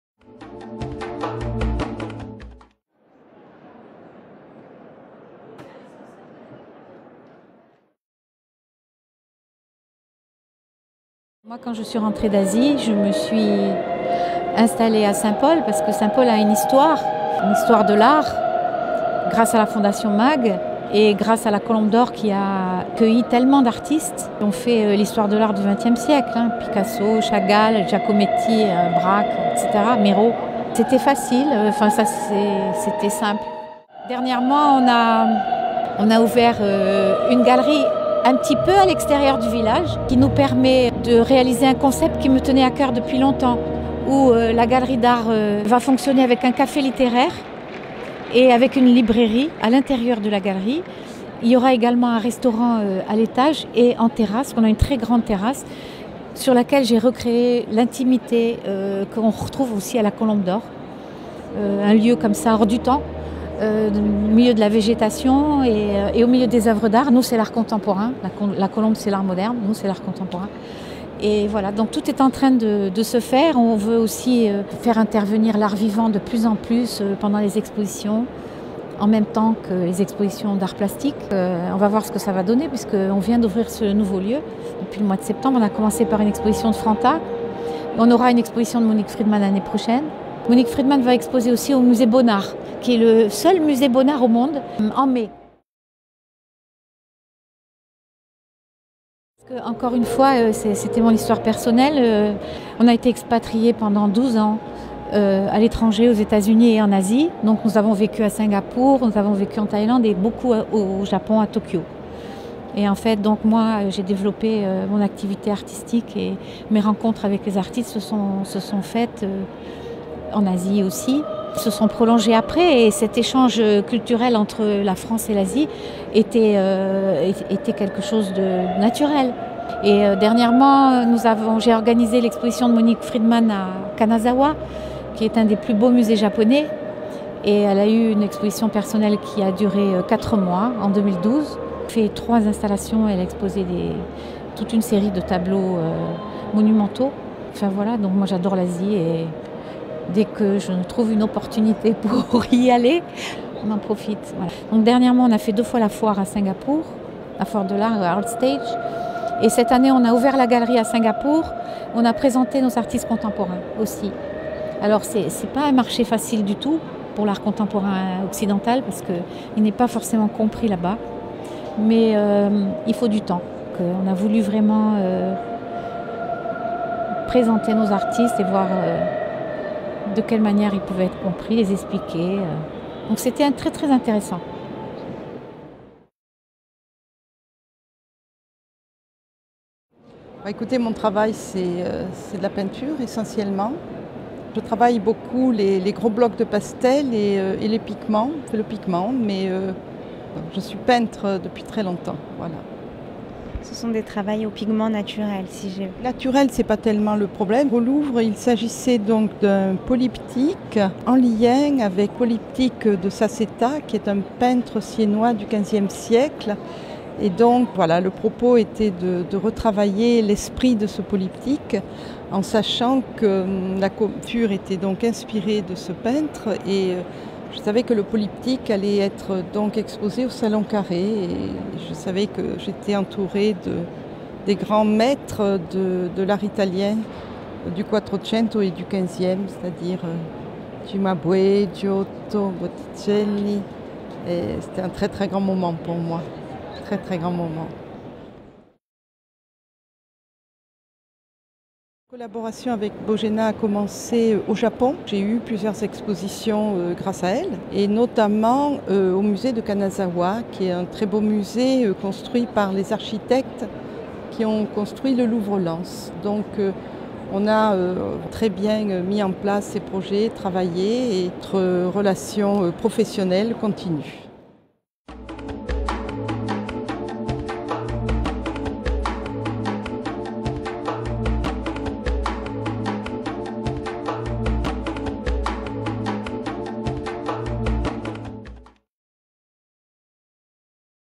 La Bogéna Galerie et Saint-Paul de Vence de Gidrol Bogéna - Art Paris Art Fair 2014
Interview de Gidrol Bogéna à ART PARIS ART FAIR au Grand Palais à Paris du 27 au 30 Mars 2014